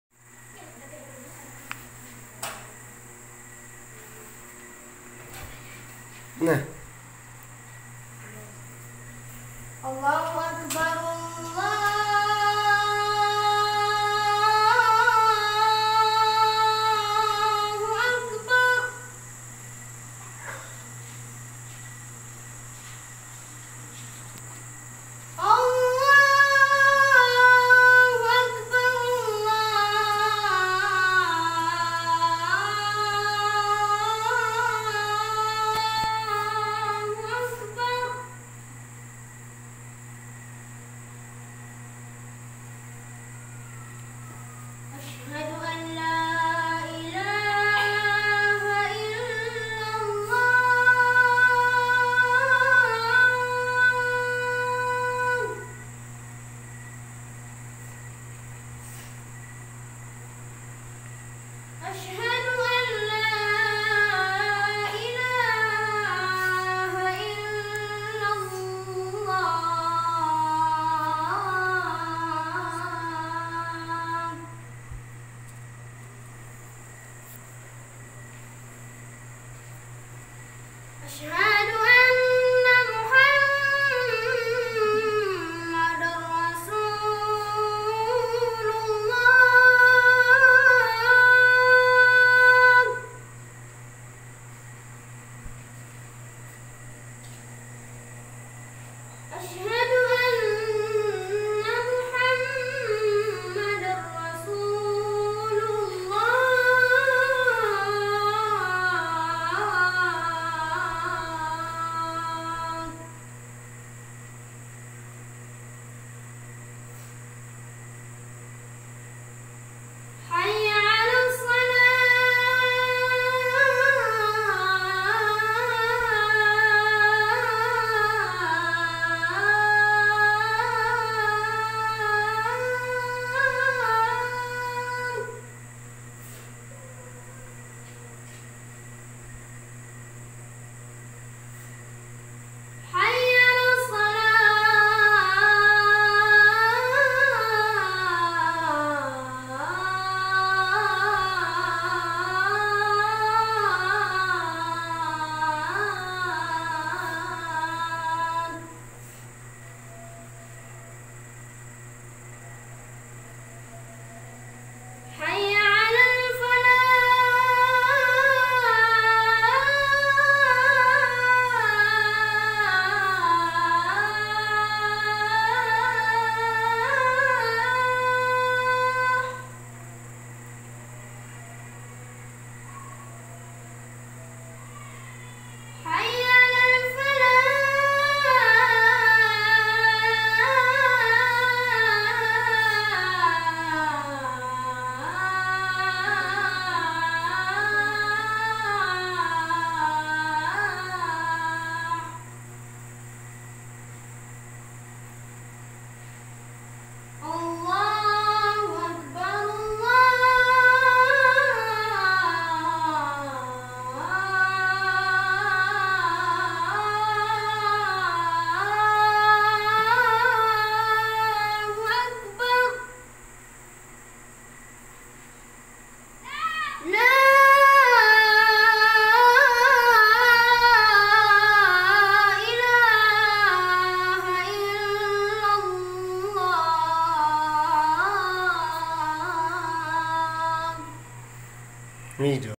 The beautiful voice adhan by IHSAN A. RADIAB
C siya po nakatira sa gannasi lanao del sur isa siyang magandang boses  matalino bata at nag Aral ng Arabic at English  kasama siya mga honor sa class Arabic at English      isa po siya maranao tribe siya